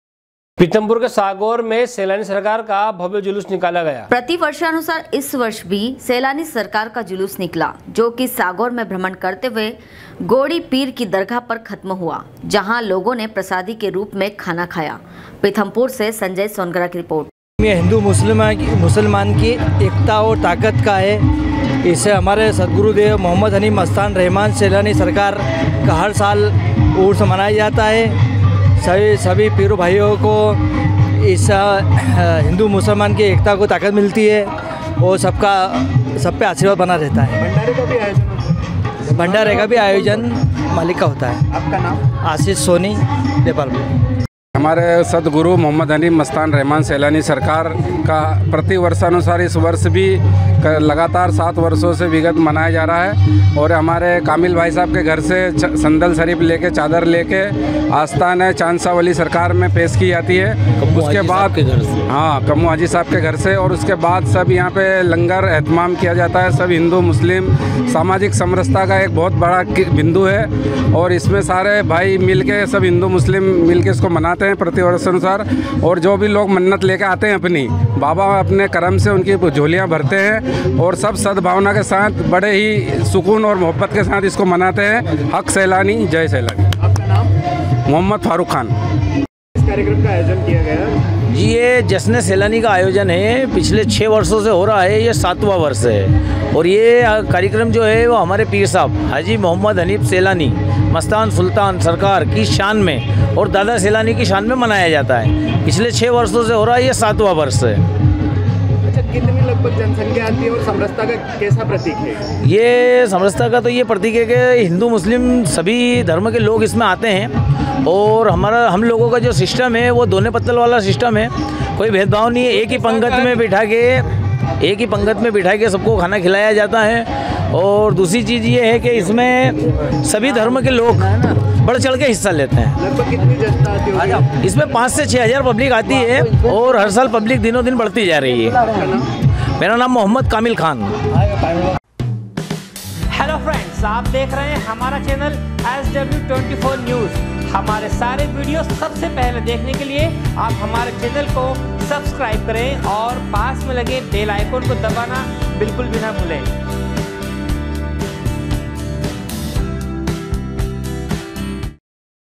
पीथमपुर मे सागौर के सैलानी सरकार का भव्य जुलूस निकाला गया
पिथमपुर से संजय सोनगरा की रिपोर्ट।